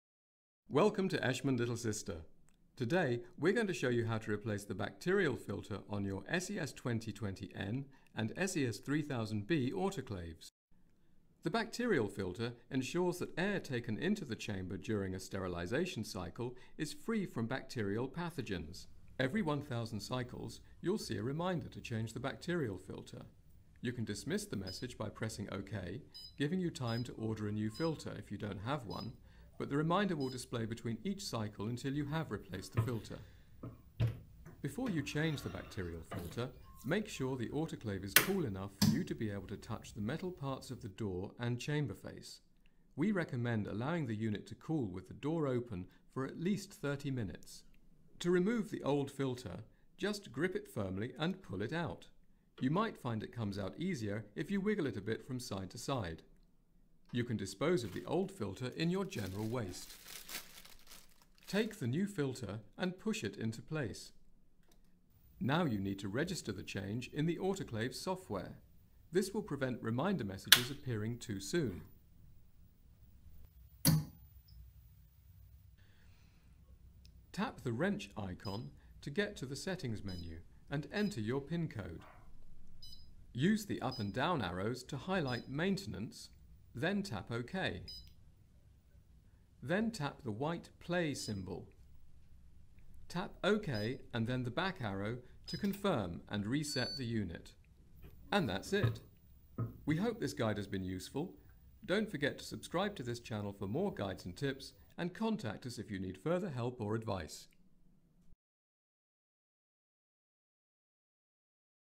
How to replace the Bacterial Filter — Eschmann Little Sister SES 3020B, SES 3000B and SES 2020N
An easy step by step guide to replacing the bacterial filter on your Eschmann Little Sister SES 3020B, SES 3000B and SES 2020N autoclaves.

Keep your surgery up and running - no need for an engineer visit!